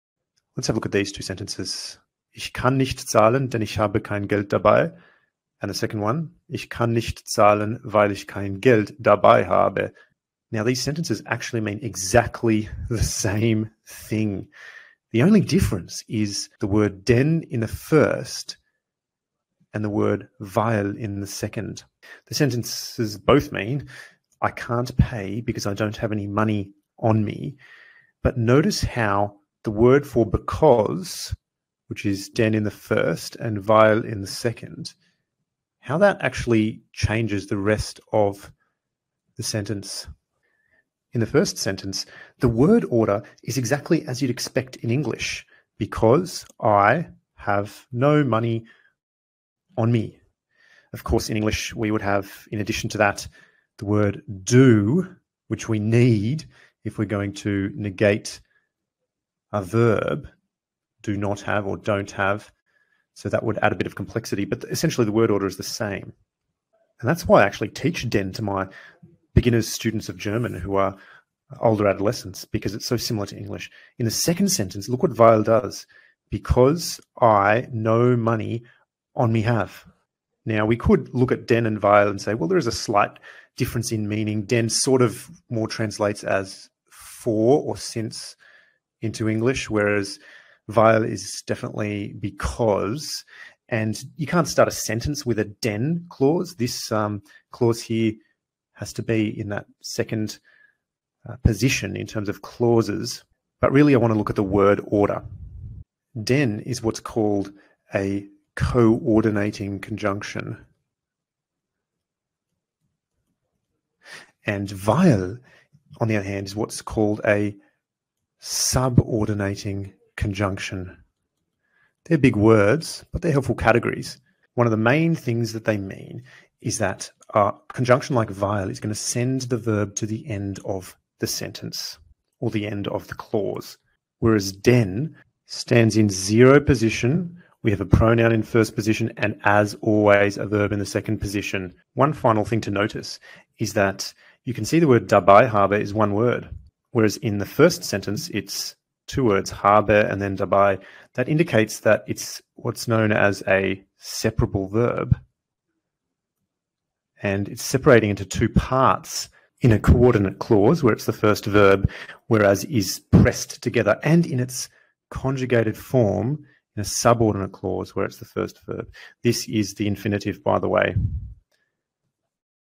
weil vs. denn | 2 minutes of German #6
This is the sixth video in a series of short videos designed to make it easy for you to keep up and improve your German. It presents two simple sentences in German, with accurate pronunciation and translation, and explains some of their key grammatical features. This video explains the important difference, especially in word order, between the conjunctions "weil" and "denn".